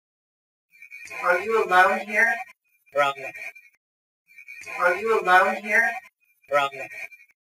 Are you alone here? Probably.
Investigator asks, "Are you alone here?" Spirit box responds, "Probably." Recorded at the Silver Lake Mill at the Delaware Agricultural Museum on October 10, 2023.